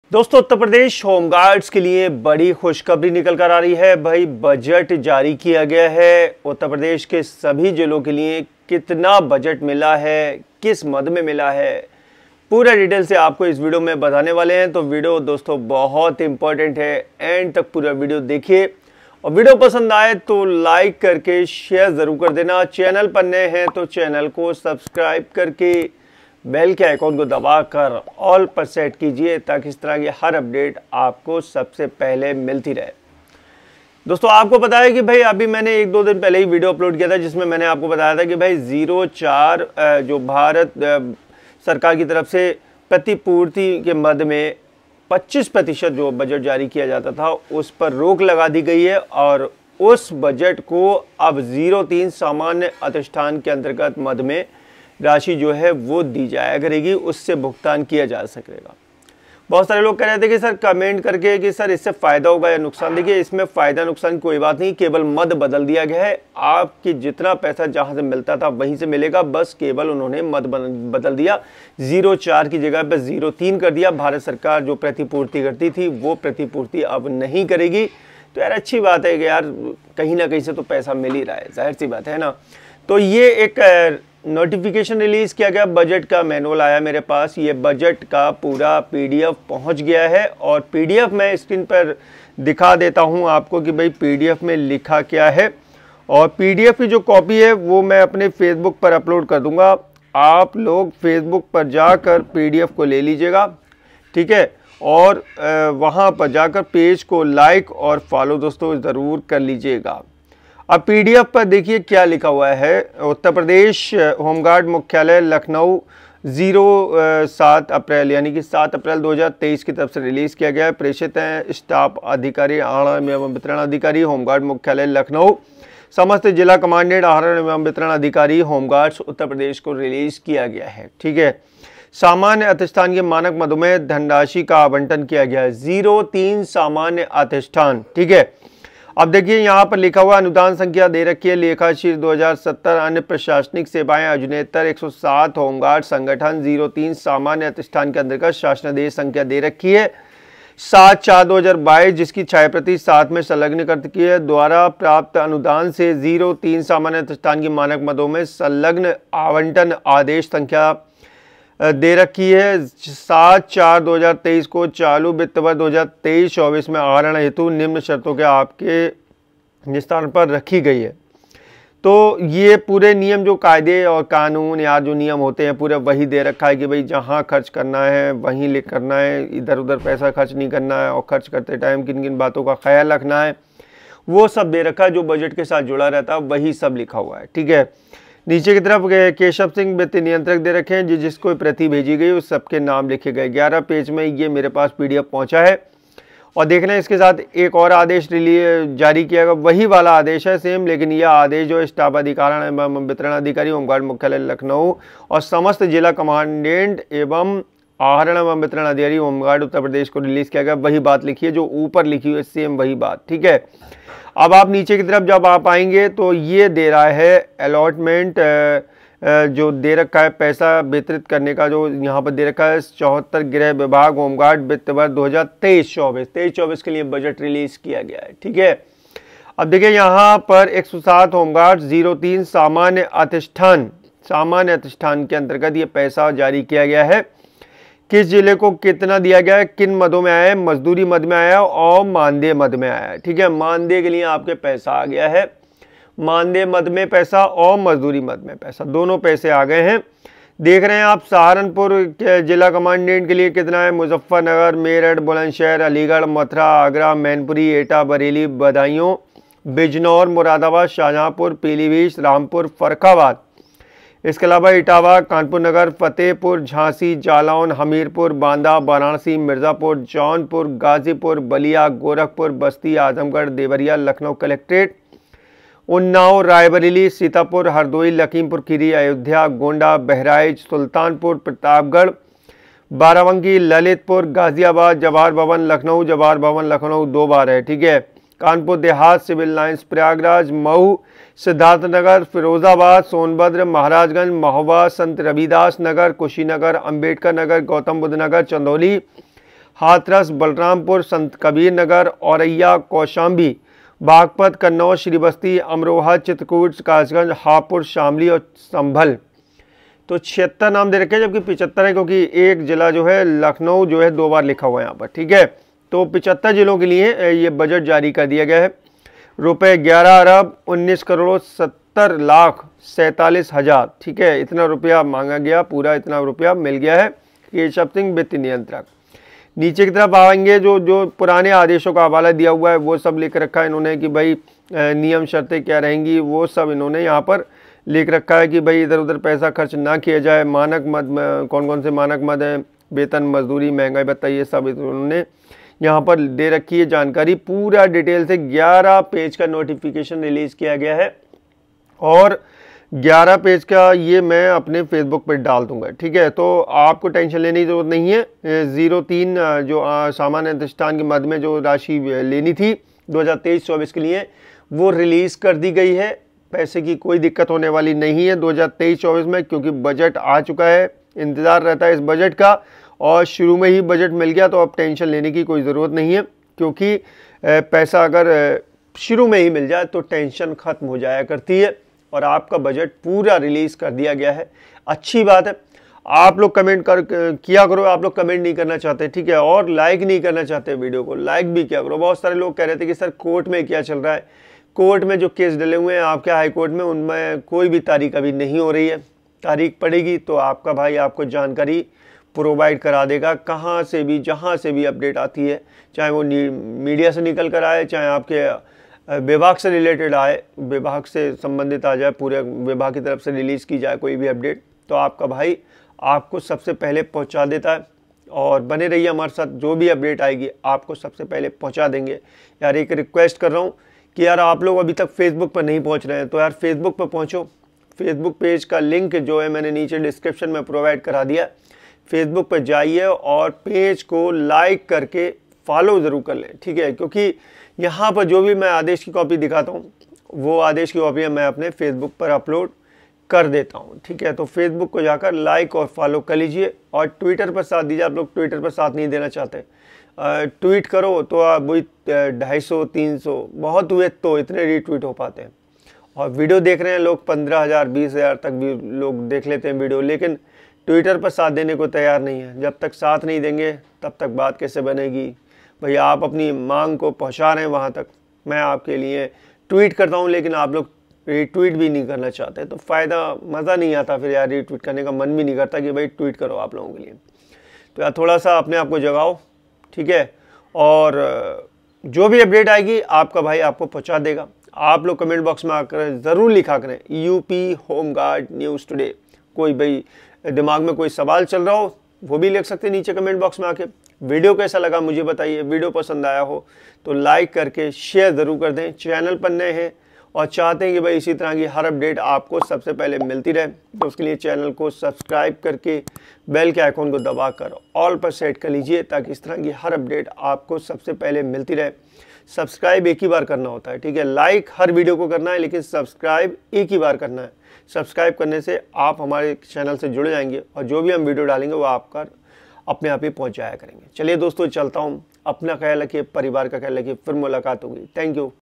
UP होमगार्ड गुड न्यूज 8 अप्रैल 2023 | UPHG बजट जारी- खाते में रूपये आएगें | UP Homeguard News Today
फेसबुक पेज से इस लिंक से जुड़े-

*My Smartphone*

*My Tripod*